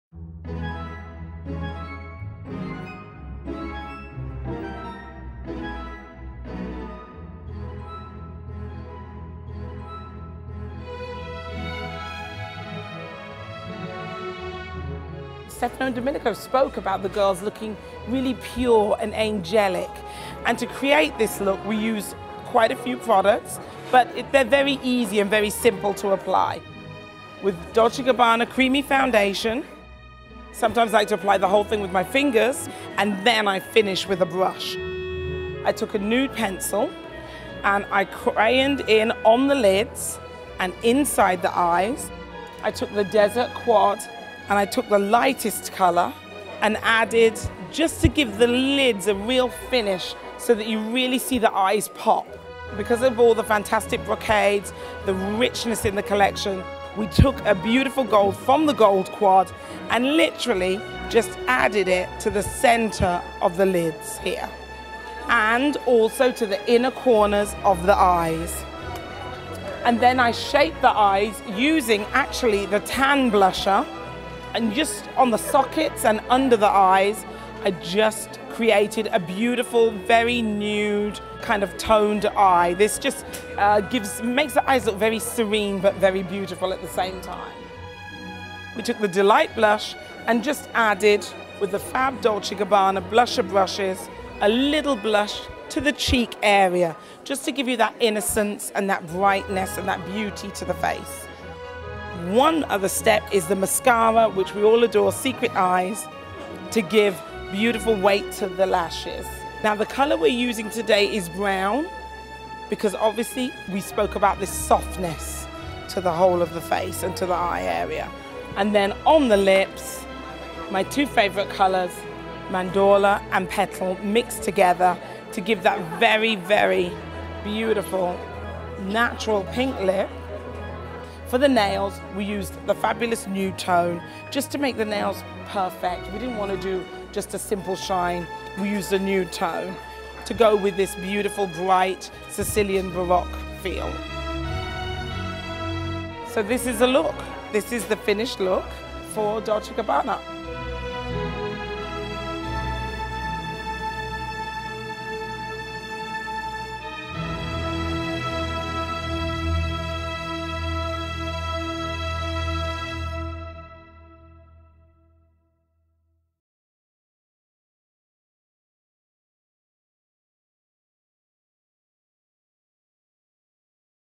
Pat McGrath for Dolce & Gabbana AW12
Get the look! Dolce & Gabbana Make Up - Milan Fashion Week VIDEO - AW 12/13 Beauty Trends Look by Pat McGrath‏